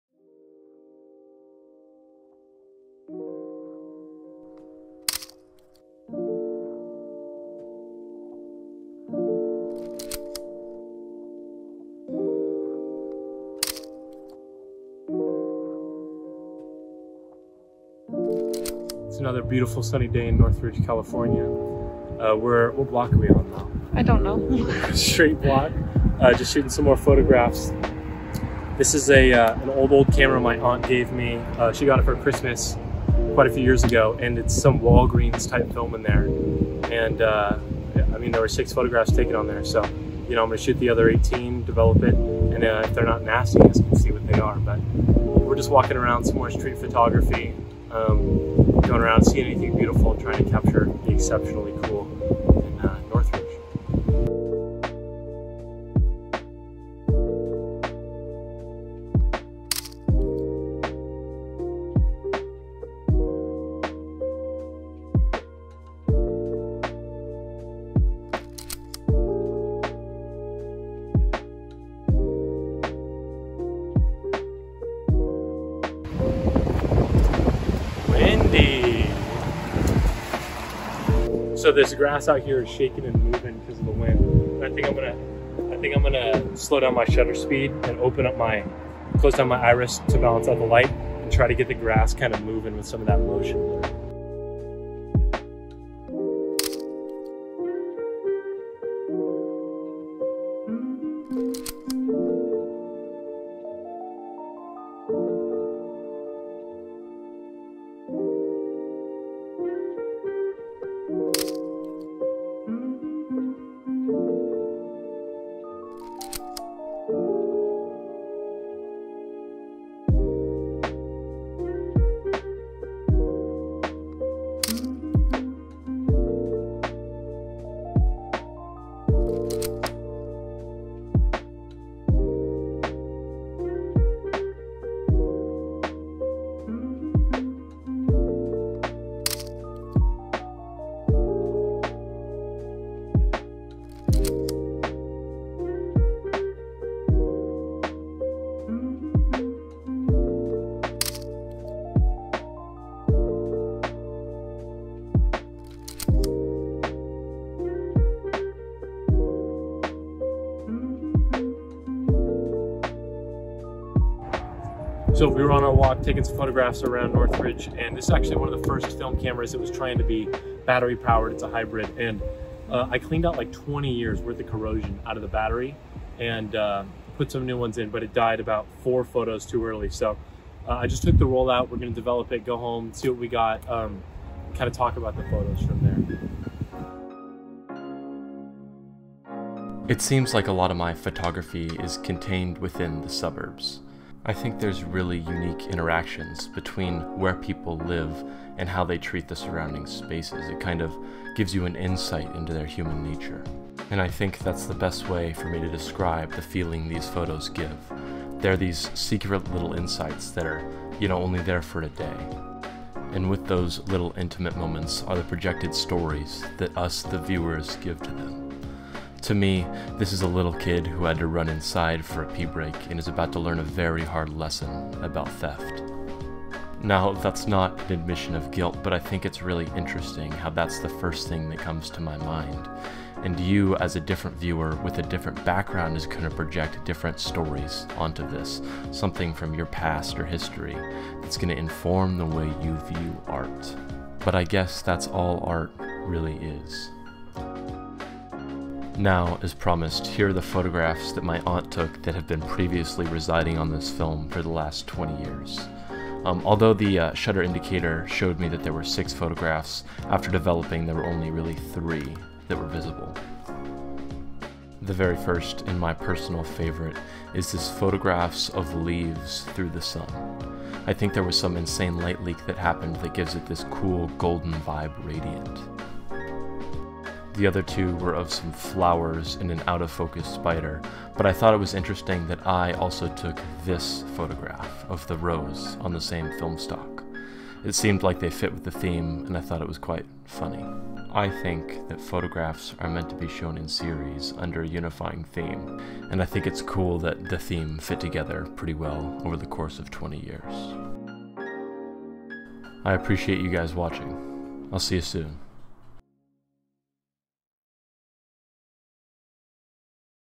Crazy Photos on 20 Years Expired Film | Nikon N2000
Photography vlog walking around Northridge with the Nikon N2000. The camera stopped working early so i missed out on some photo opportunities. I appreciate you guys watching and I hope to make more videos. Shooting for one every week.